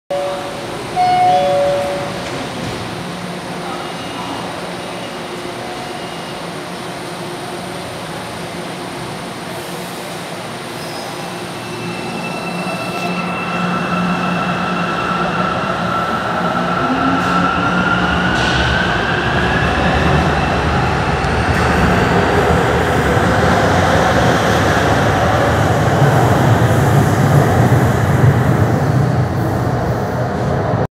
Série 400 (412) partindo da estação República
Série 400 (412 - Fase 1) (Hyundai Rotem) partindo sentido Luz